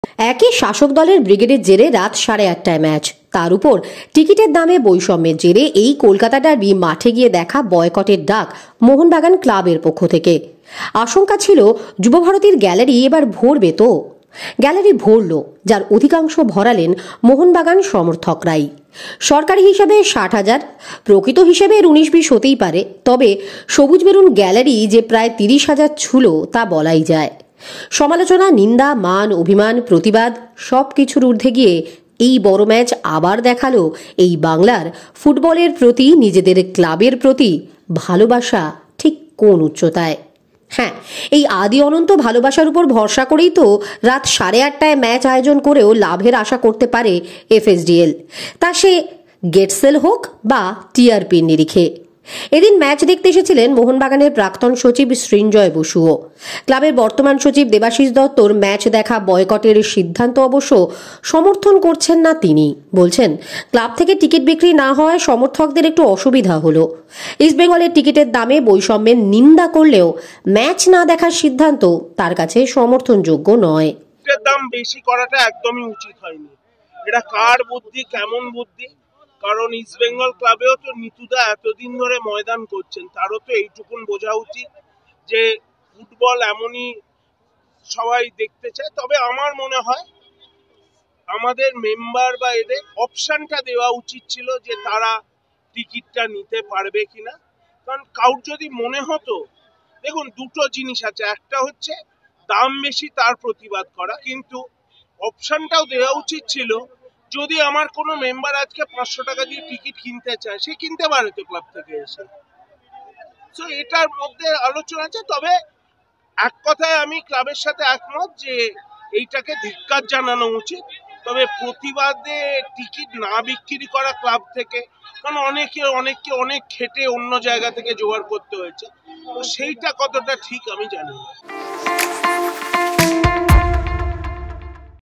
boycott প্রশঙ্গে বর্তমান মোহনবাগান সচিবের সঙ্গে মতবিরোধ প্রাক্তন সচিবের
mohunbagan mohunbagansupergiants indianfootball mbsg শাসক দলের ব্রিগেড, ম্যাচ দেখা boycott এর ডাক, সবকিছু ছাপিয়ে kolkataderby তে ভর যুবভারতীর gallery , সৌজন্যে অনেকটাই mohunbagan সমর্থকরা। এসেছিলে mohunbagan এর প্রাক্তন সচিব সৃঞ্জয় বসুও। eastbengal এর টিকিটের দাম নিয়ে বৈসম্যের নিন্দা করলেও boycott কে সমর্থন জানাচ্ছেন না , বললেন সৃঞ্জয়।